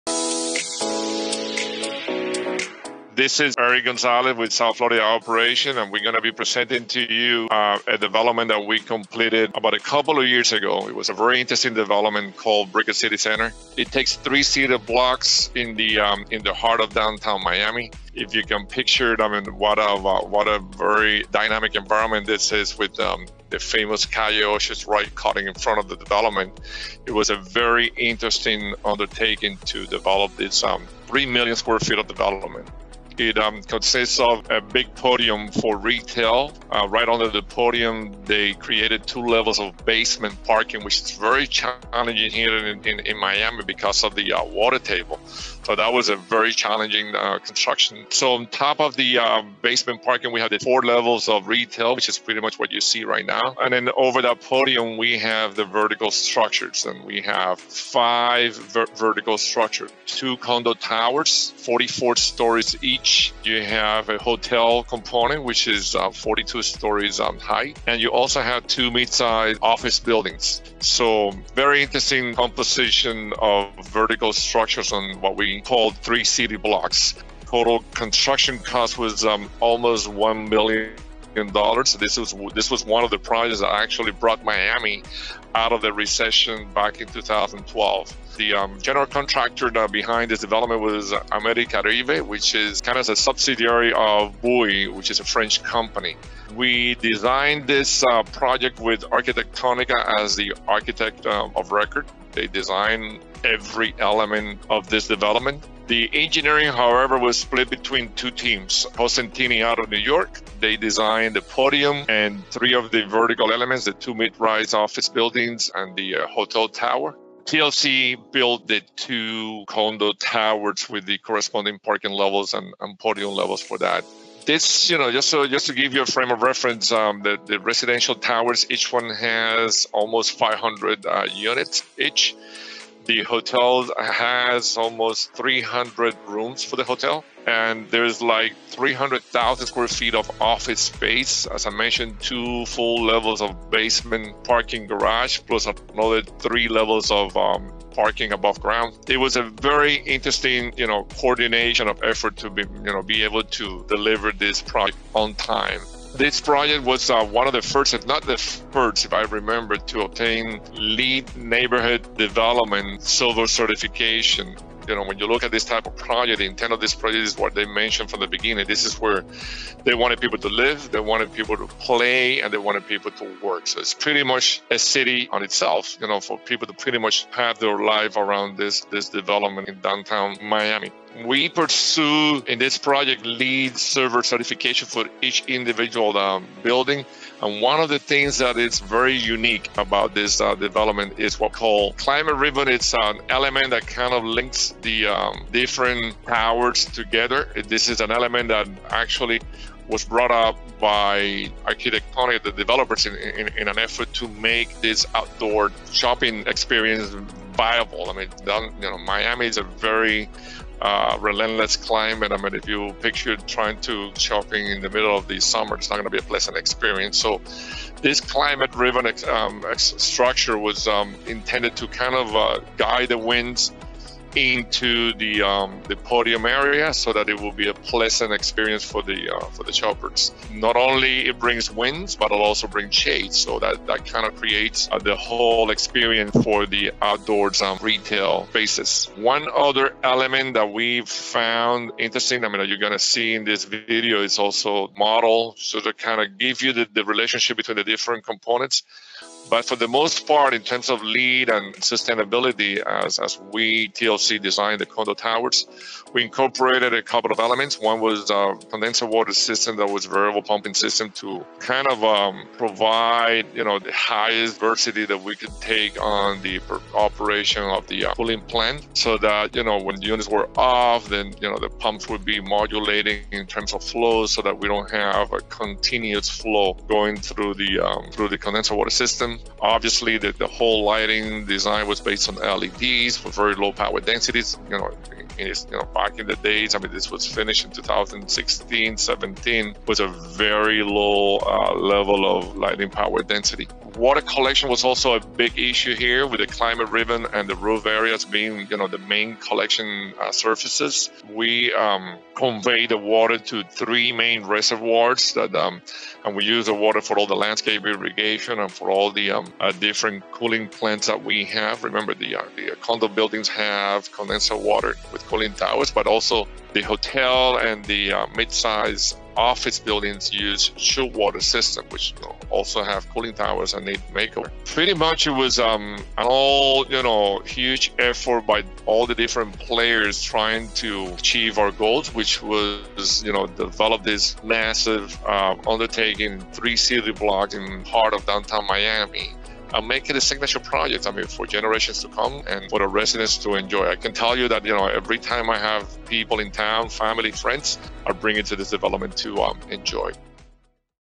Virtual Field Trip Friday - Brickell City Centre
In celebration of Eweek2021, we are highlighting the amazing Brickell City Centre for our Field Trip Friday!